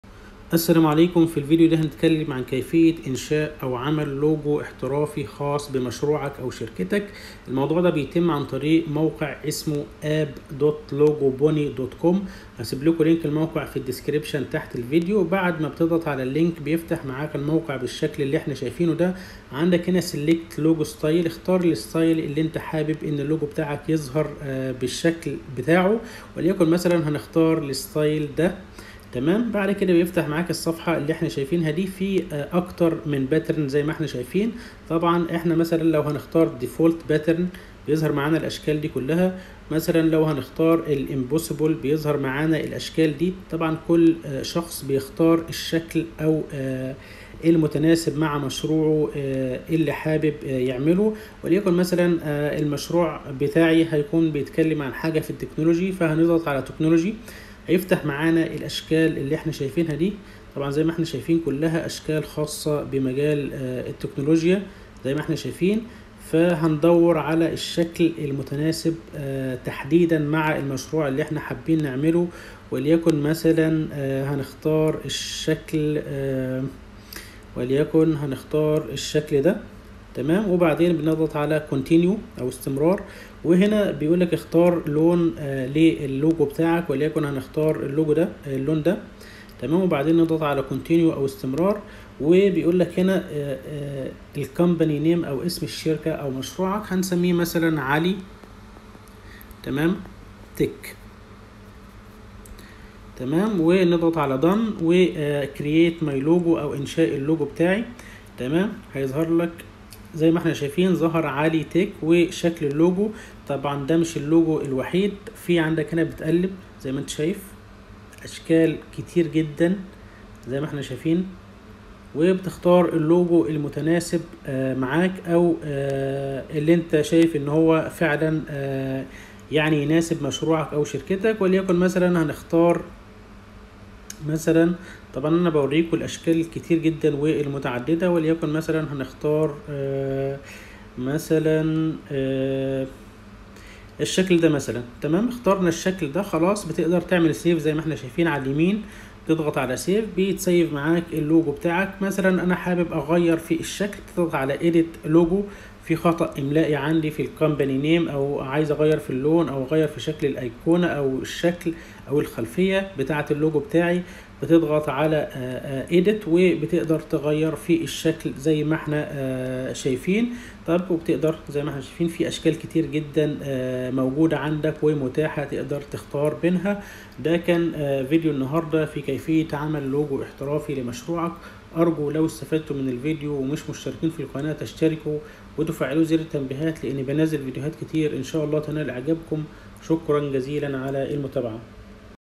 عمل شعار إحترافي في دقيقتين !! Logo Design
طريقة عمل لوجو احترافي في دقيقتين وبمنتهى السهولة

FOR BUSSINESS INQUIRES: